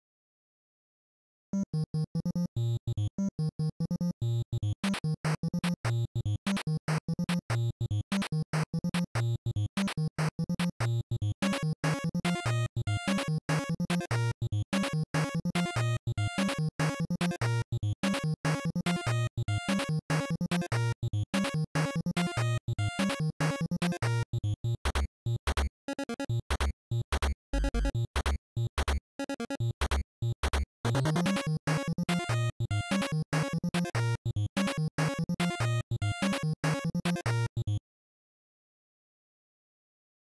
KORG Gadget MM2 Inspired Song
Quick (sub 1 hr) composition inspired by the legendary Manami Matsumae